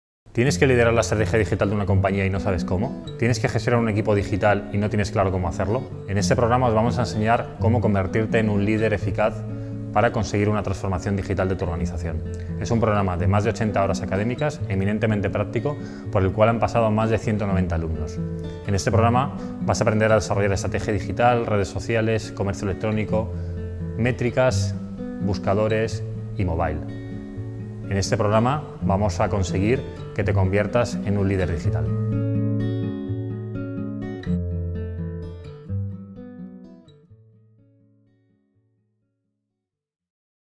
ISDI | DIBEX | Digital Business Executive Program
Formación para directivos y managers de empresas que necesitan reciclarse para incorporar el entorno digital de una manera funcional en su día a día. Más información